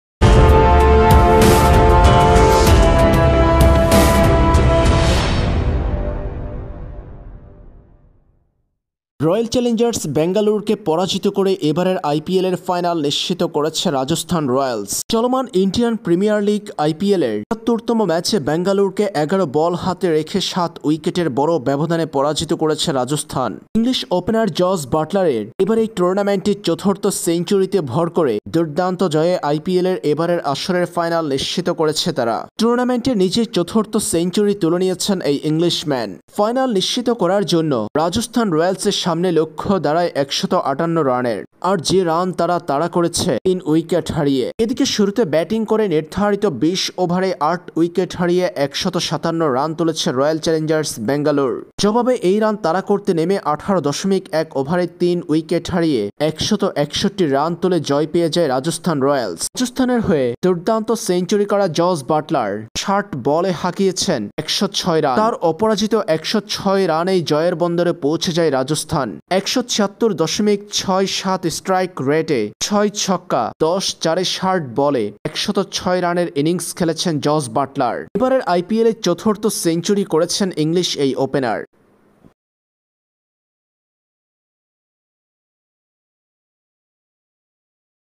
জশ বাটলারের বিস্ফোরক সেঞ্চুরি💥ছক্কার ঝড়❗ব্যাঙ্গালোরকে উড়িয়ে ফাইনালে রাজস্থান | RR vs RCB | Buttler
Jos buttler century ipl 2022 - iplt20 2022 | IPL 2022 - Indian Premier League 2022 - Rajasthan Royals vs Royal Challengers Bangalore ipl 2022